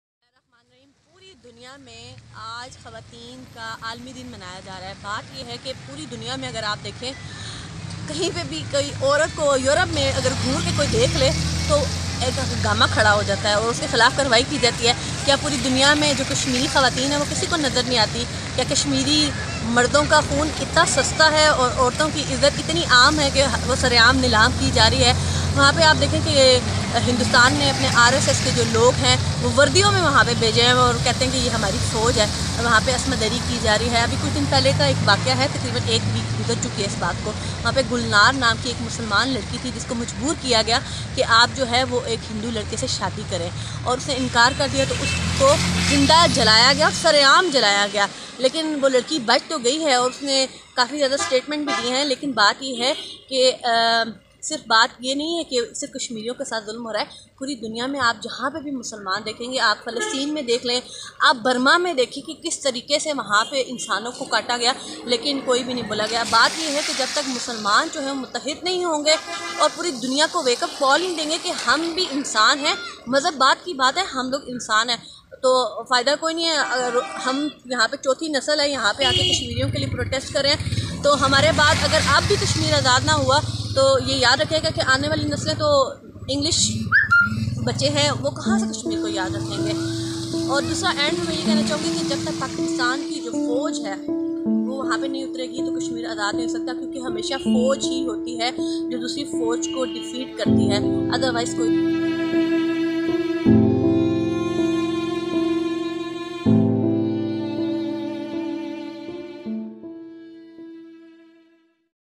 خواتین کے عالمی دن پر کشمیری خواتین کو بھولا نہی جا سکتا
آج خواتین کا عالمی دن منایا جا رہا ہے کیا کشمیر کی خواتین کے حقوق نہیں ہے کشمیر خواتین کی عزتیں سر عام نیلام ہو رہی ہیں ان کا خون بہایا جا رہا پھر دنیا کون سے خواتین کے حقوق کی بات کر رہی ہیں
کنول حیات